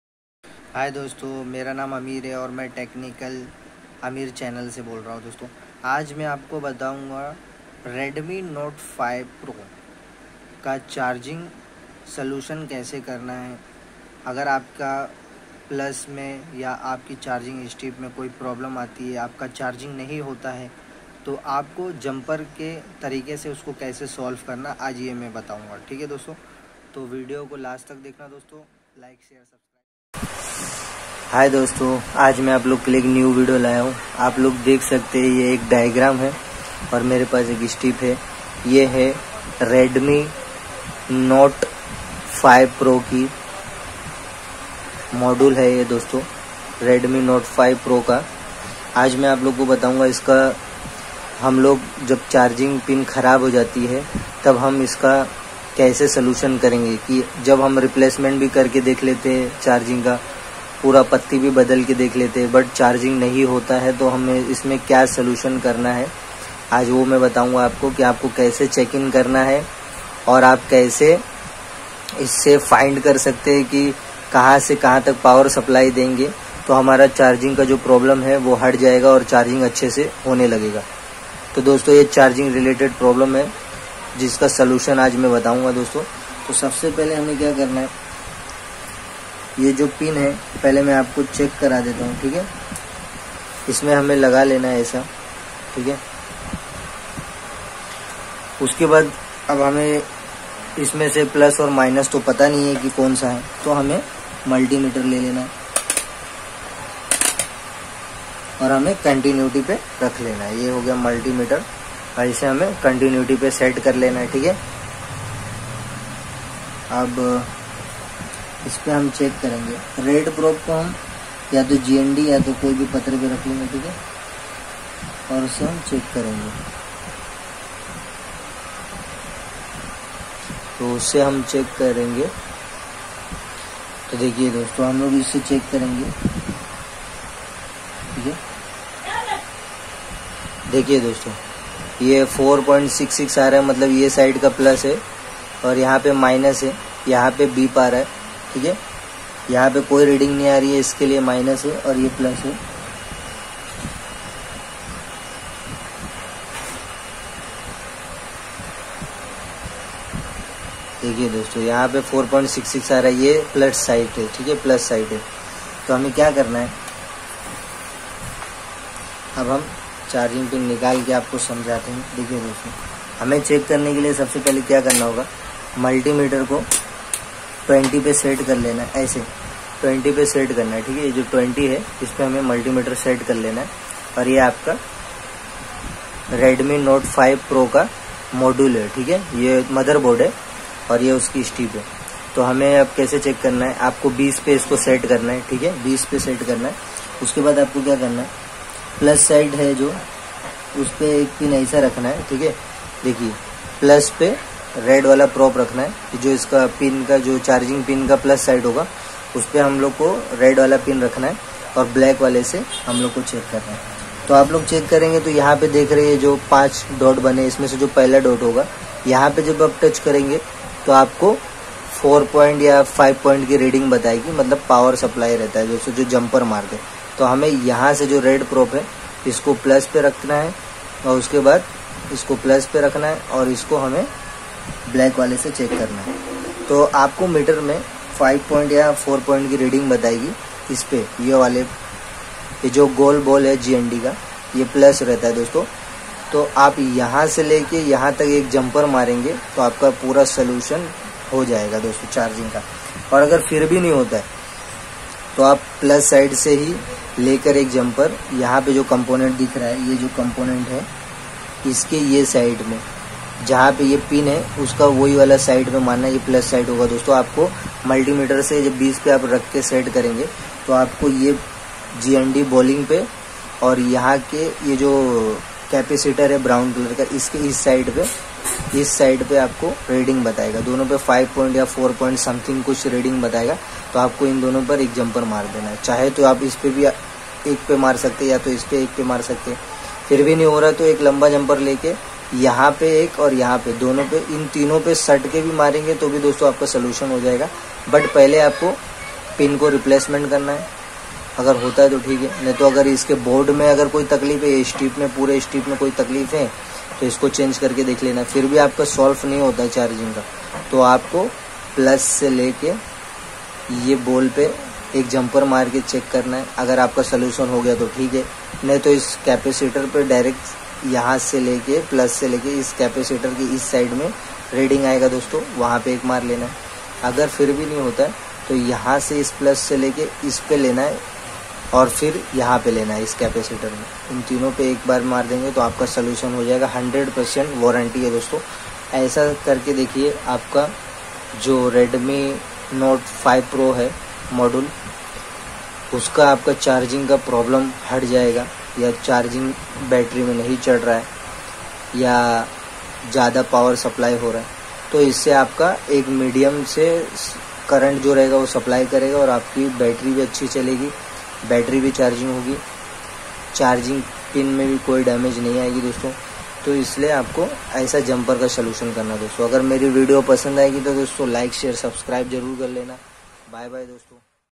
Redmi note 5 pro charging, battery, related any problems solution with diagram and supply checking
Hy dosto Redmi note 5 pro charging, battery related any problems solution with diagram and supply checking full details this video please watching the video end .